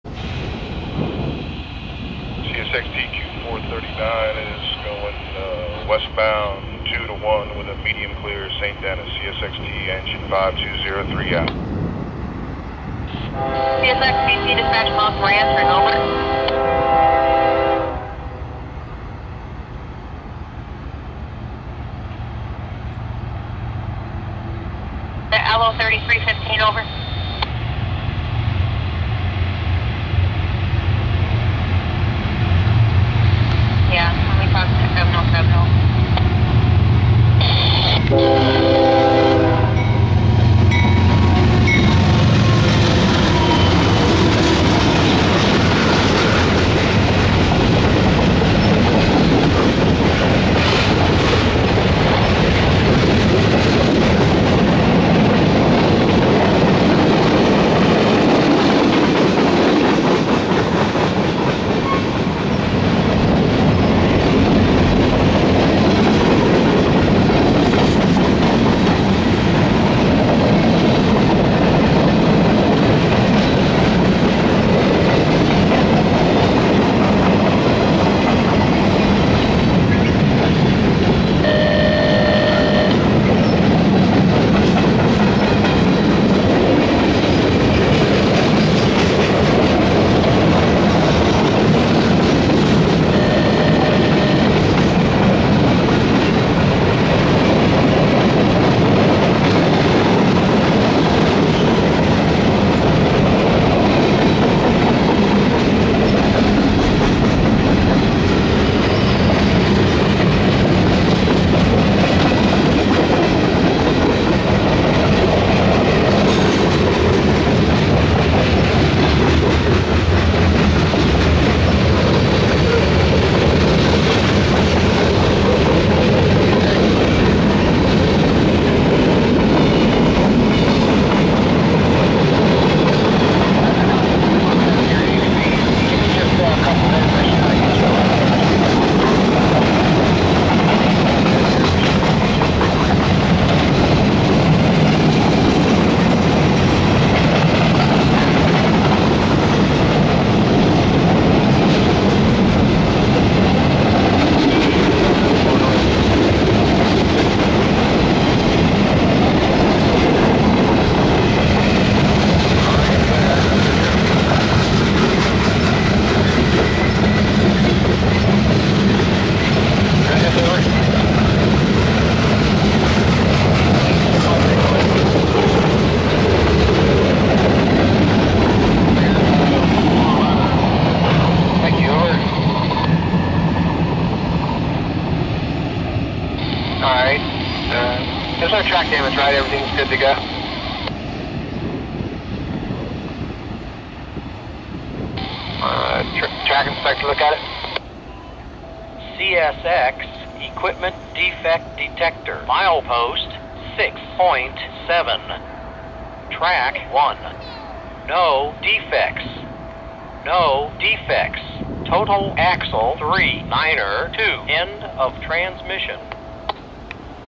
CSX Train With Various Rolling Stock & Graffiti
★ I AUTOMATICALLY ACCEPT ALL VIDEO RESPONSES ON ALL MY VIDS! ★CSX Q439 In St Denis, Maryland